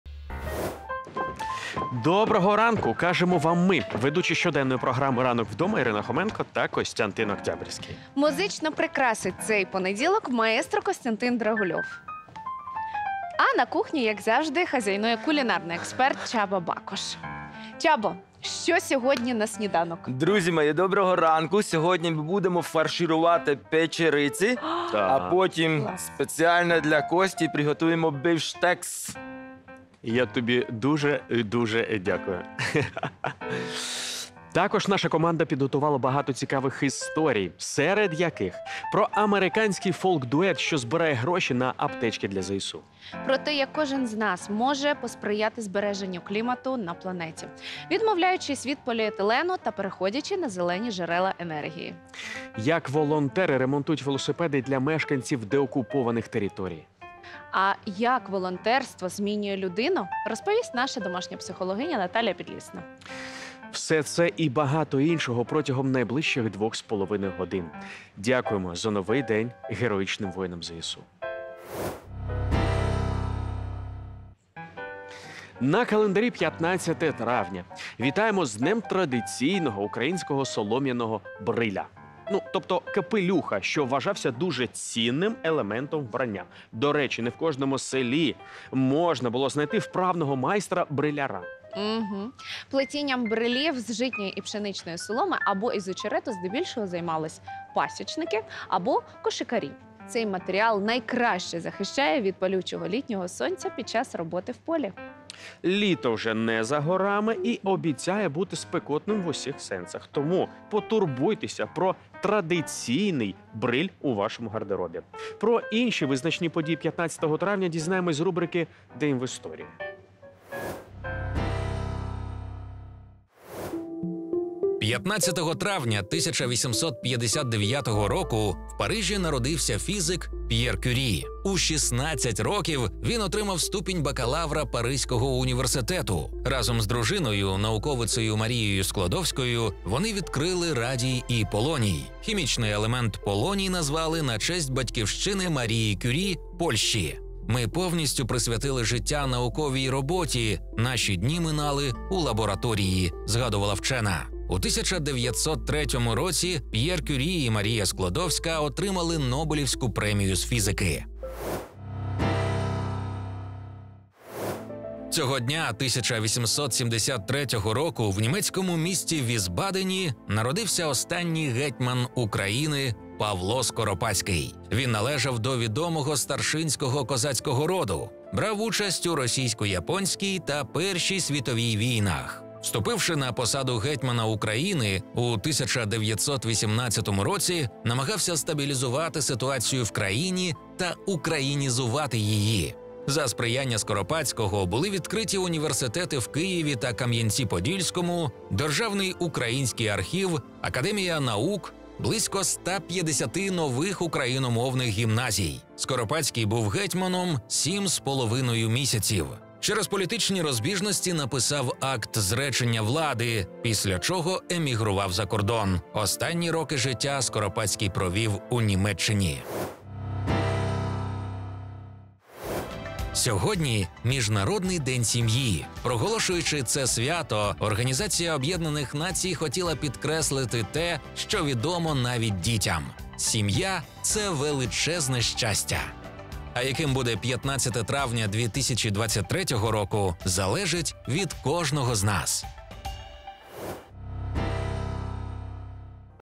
День народження Павла Скоропадського: 15 травня в історії
Щоденна програма "Ранок Вдома" починається! На календарі 15 травня, тож дізнаймося, яким цей день був в історії.

15 травня 1859 року в Парижі народився фізик П’єр Кюрі. Разом із дружиною-науковицею Марією Склодовською вони відкрили радій і полоній. Цього дня 1873 року в німецькому місті Вісбадені народився останній гетьман України Павло Скоропадський. Він належав до відомого старшинського козацького роду, брав участь у Російсько-японській та Першій світовій війні. Сьогодні – Міжнародний день сім’ї. Проголошуючи це свято, організація ООН хотіла наголосити те, що сім’я – це величезне щастя.

Більше про важливі події цього дня дізнавайтеся у матеріалі програми "Ранок Вдома".